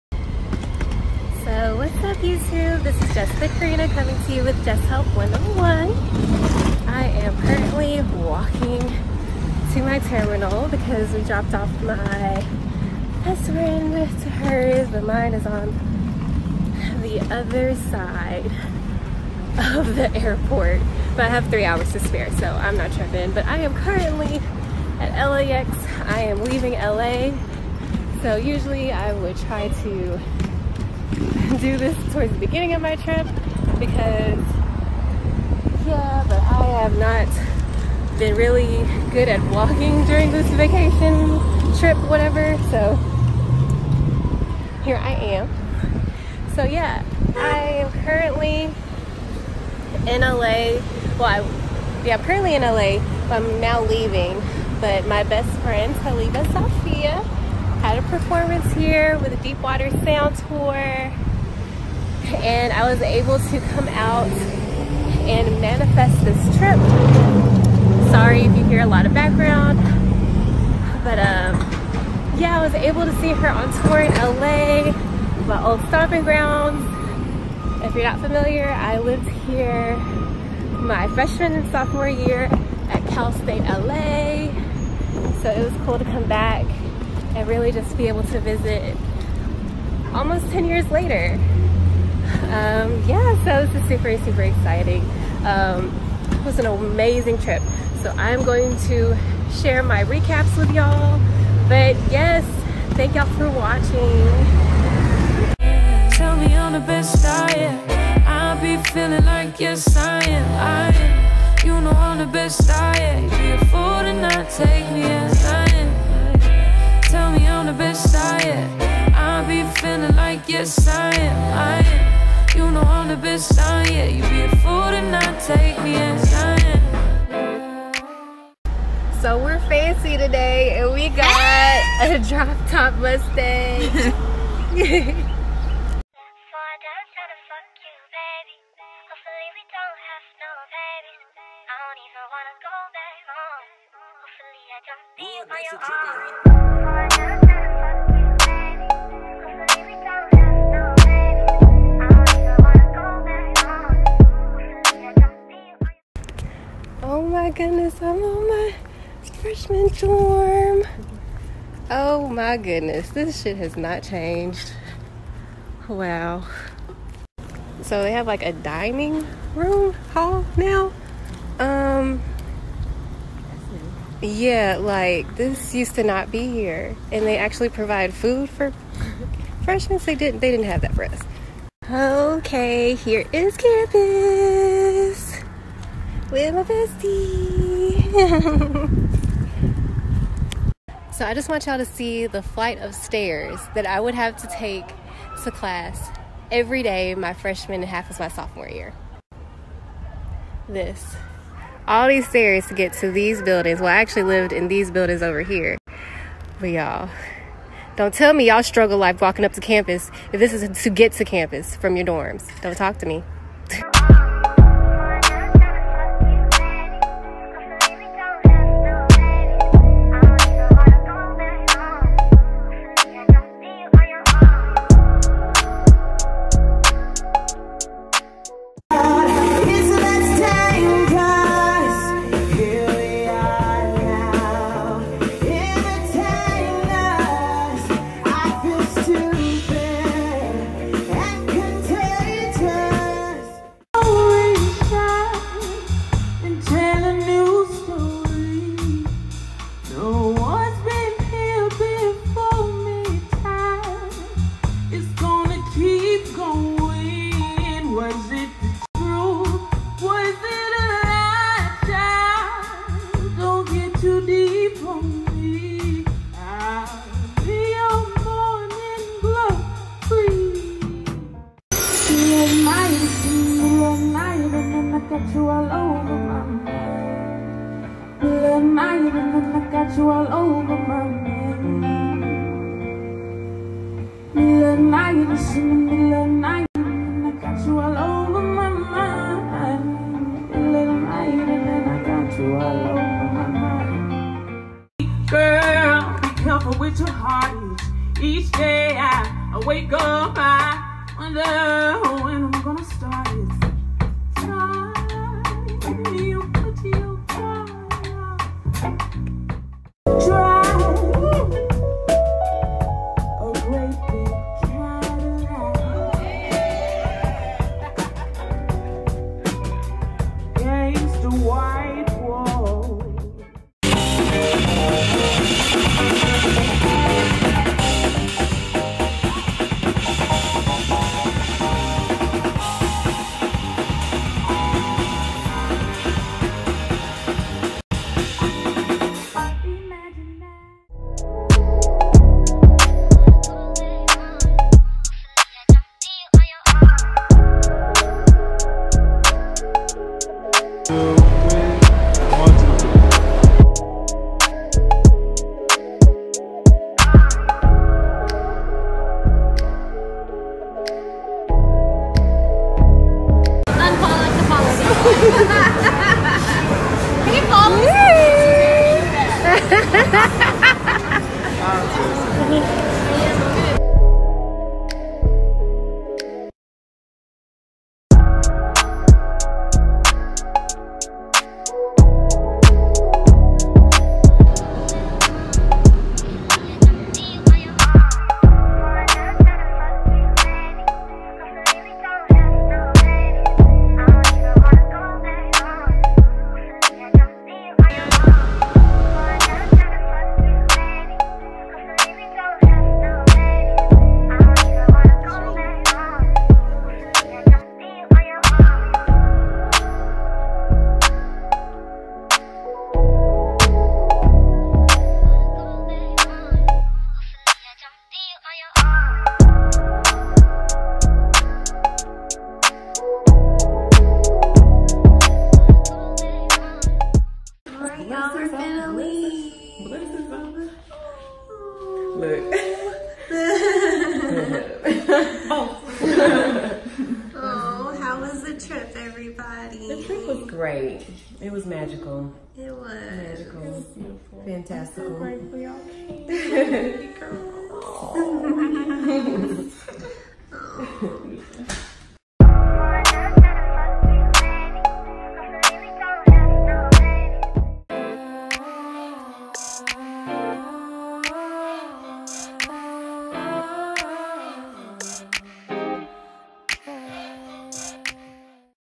SUMMER 2019: LOS ANGELES | Jess Help 101
Recorded: July 19-23, 2019

Here is my last vlog from my summer social media hiatus! My previous vlog was about manifestations and here is an example of being able to visit LA for a weekend getaway! I’ve been wanting to go back to LA for awhile now but I never made plans to actually go. I found out ADeepWaterSoundTour was making a stop there so I made up my mind last minute to find a way! When I say the arrangements for flights and where to stay just lined up perfectly... I manifested a trip for just about free😫😭 (just had to pay for food, transportation, and souvenirs)! I posted most of the clips on my Instagram stories, but here’s a recap if you missed it!